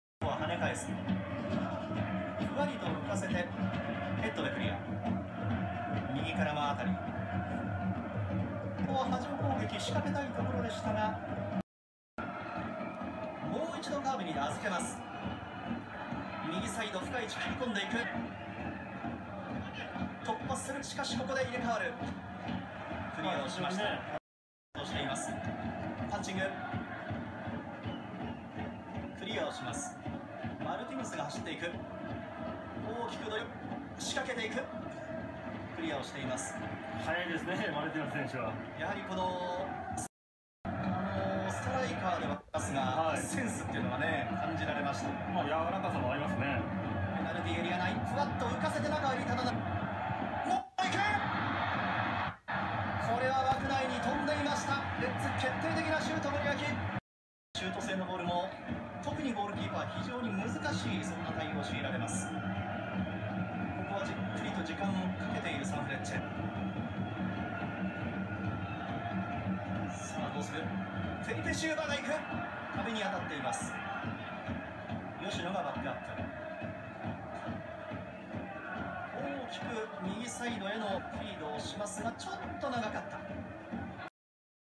ธีรศิลป์ลงแค่ 45 นาที ซานเฟรซเช่จบไม่ลงเจ๊าอุราวะจืด 0-0
ซานเฟรซเช่ ฮิโรชิม่า vs อุราวะ